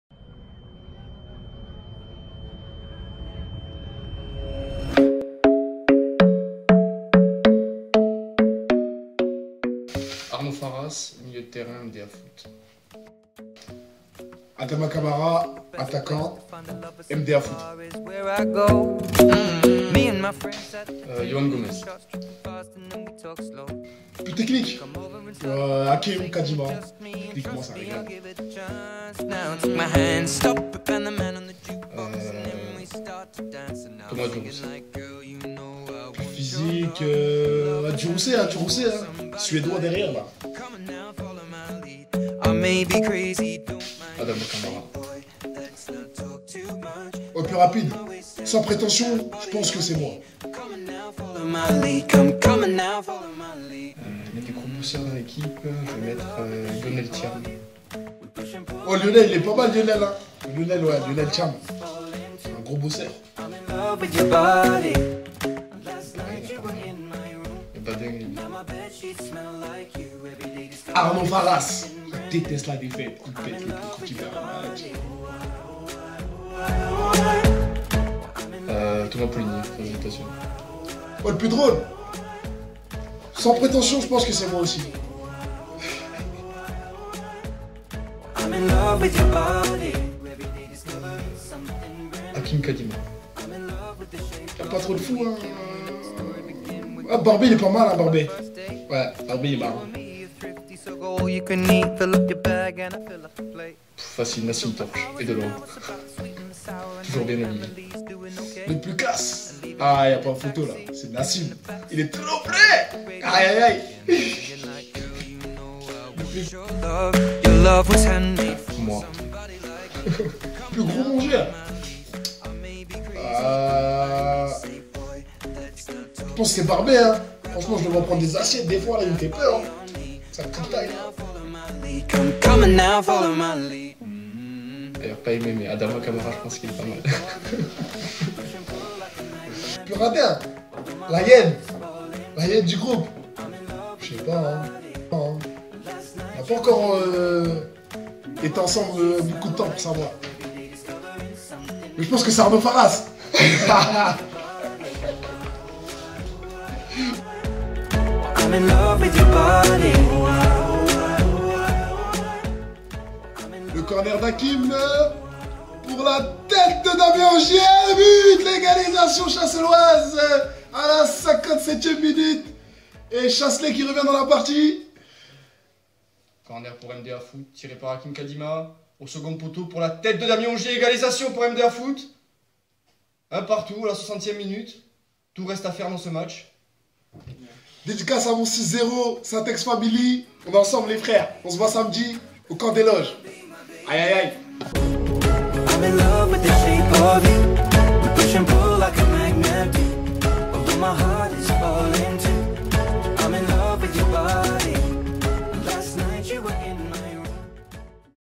MDA Le Plus
Arnaud Farras et Adama Camara se sont prêtés au jeu cette semaine.
Apprenez en plus sur l'effectif N2 de MDA Foot grâce à eux !